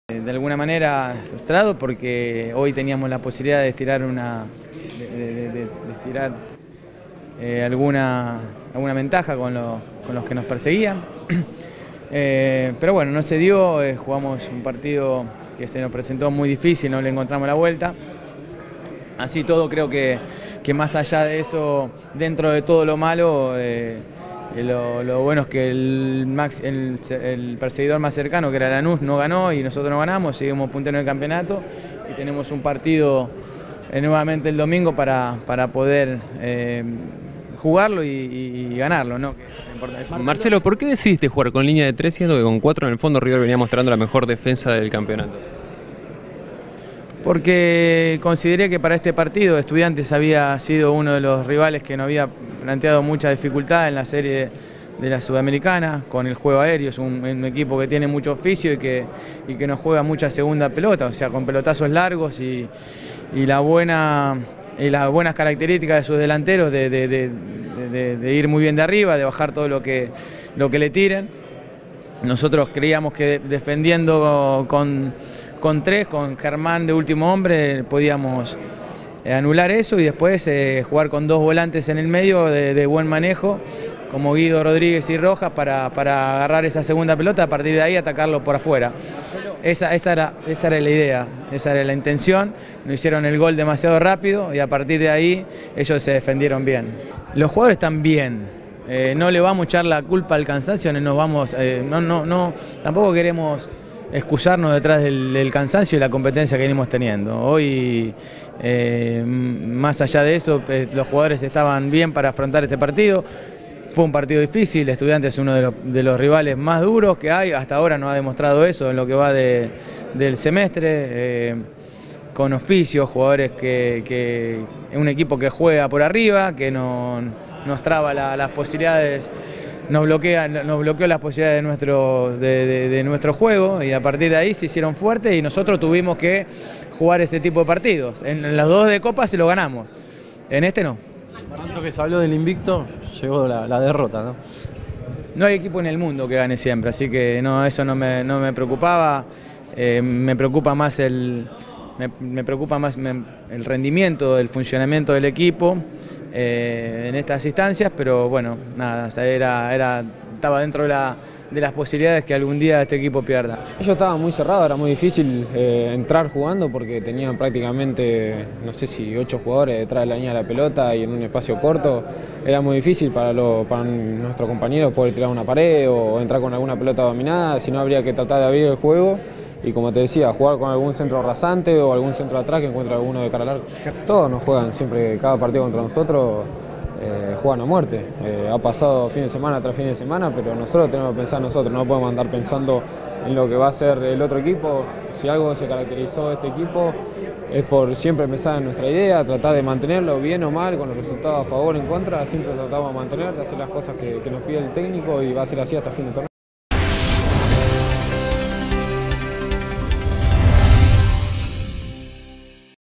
"Nada nos perturba para afrontar la recta final", aseguró el director técnico Marcelo Gallardo
El entrenador de River Plate, Marcelo Gallardo, explicó que "nada" los "perturba para afrontar la recta final", tras perder un invicto de 31 partidos ante Estudiantes La Plata, que lo derrotó por 1 a 0, de cara a la definición del torneo de Primera División donde su equipo está puntero y a las semifinales en la Copa Sudamericana ante Boca Juniors.
“No deja de ser un partido, hay que darle la importancia que merece la derrota y aprender de eso. No habrá nada que nos perturbe para afrontar la recta final. No hay equipo en el mundo que gane siempre, ahora me preocupa el rendimiento del equipo", destacó el 'Muñeco'.
Luego, Gallardo manifestó: “Hoy pensábamos en ganar este partido para estirar la diferencia. Nos faltaban jugadores importantes, jugaron jóvenes que están dando sus primeros pasos y es bueno que se sientan respaldados. El desgaste es superior y después lo pagás, porque jugamos muy seguido”.
“Los que vienen jugando lo vienen haciendo con continuidad, los chicos vienen acompañando, por eso es importante que tengan confianza y apoyo. Me tengo que enfocar en lo que tenemos y presentar a los jugadores que mejor estén", completó.
Después, el entrenador reiteró: "Hay que pensar que no tenemos demasiado como para rotar, pero los que vienen jugando lo vienen haciendo con continuidad. Es bueno apoyar a los jóvenes y que se sientan respaldados. Tienen que asumir el rol".
El plantel se entrenará mañana, a partir de las 17, en el estadio Monumental de Núñez, con atención a la prensa.